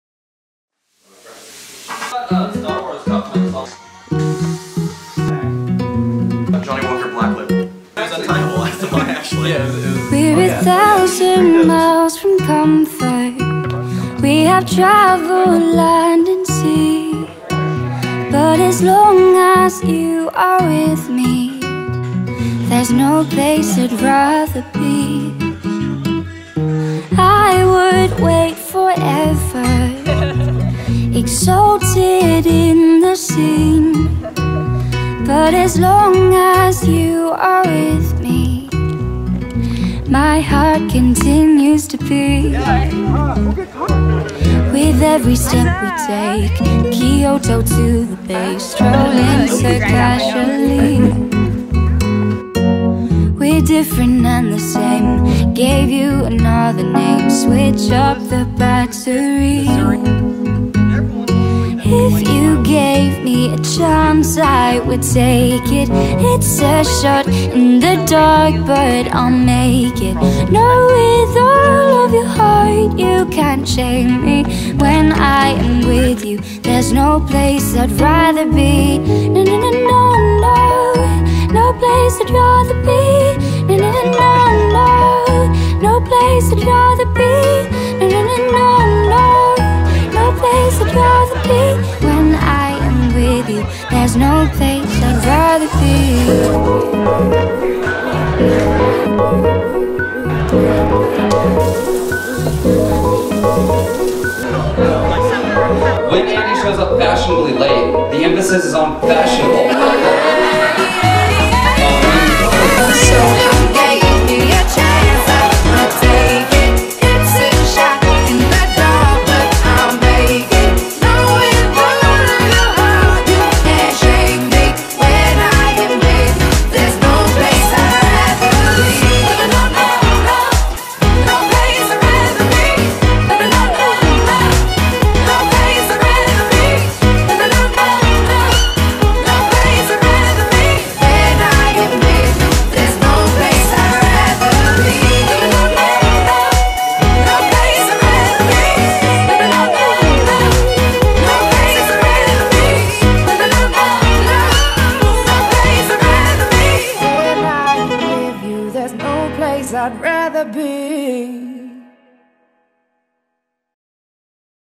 Andy and Kim Wedding Video
This was my first time shooting video at a wedding while I was photographing it! I made this video as a little surprise for the couple :)  Congratulations Andy and Kim!